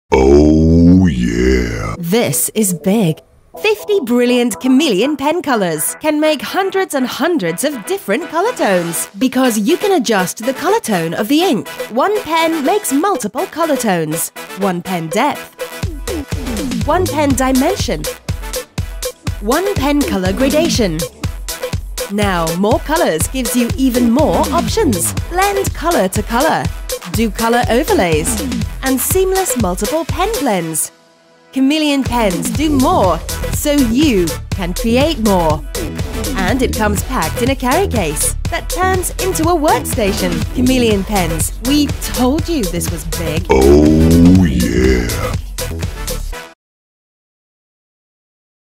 Chameleon Art Products - 52 Pens
A marker pen that lets you change the color tone of your ink!
Imagine a world where you no longer need multiple pens to create a color gradation... well, that world is real!

Using just one Chameleon Pen you can create smooth color gradations in a matter of seconds.